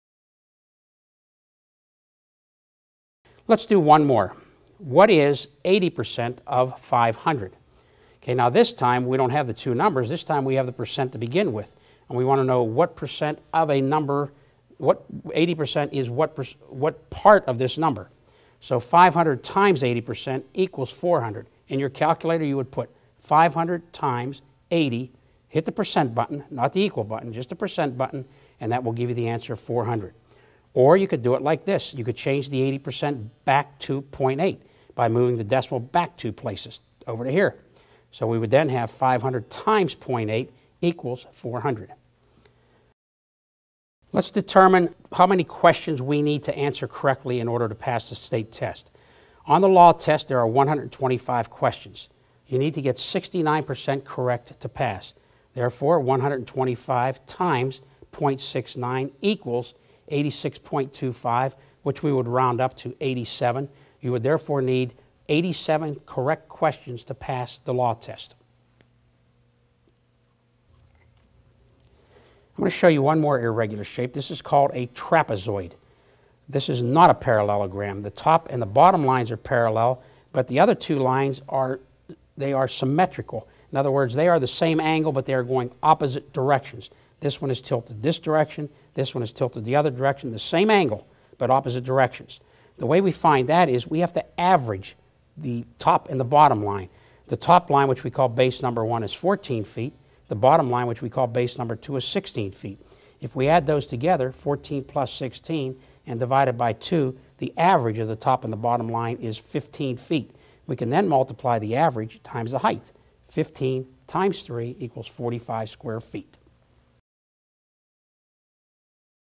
Construction Math - English
Do you need to prepare for your contractor's license exam? Are you confident you have the necessary math skills in order to pass? Contractor's State License Service has helped over 100,000 contractors successfully obtain their contractor's license for over 23 years. This clip offers a glimpse of the English Construction Math class available in live instruction and through DVD.